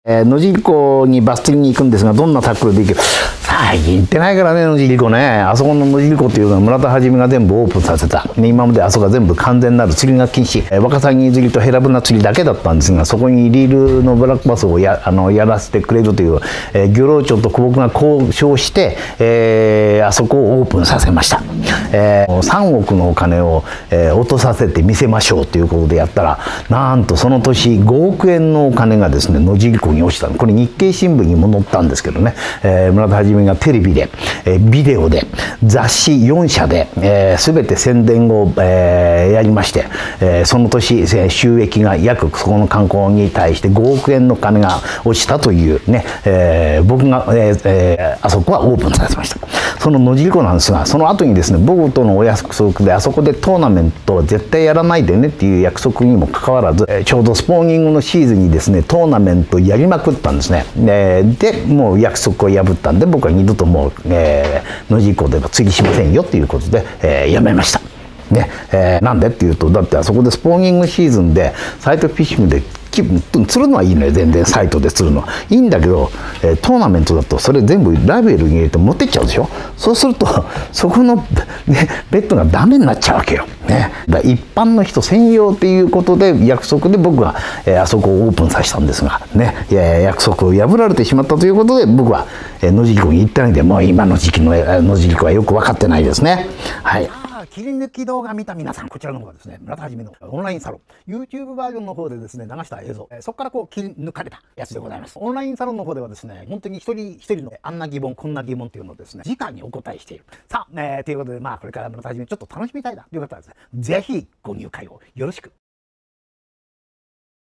【村田基】「約束が違いますよね？」野尻湖では二度と釣りをしません。村田さんが野尻湖に行かなくなった理由とは？
スモールマウスバスが有名の野尻湖。
村田さんも昔はよく行っていたようですが、野尻湖では二度と釣りをしないと決めているようです。それはなぜでしょうか？
今回は村田さんが、野尻湖に行かなくなった理由について語っています！

　生配信先！
〇潮来釣り具センター
　ジムのお店！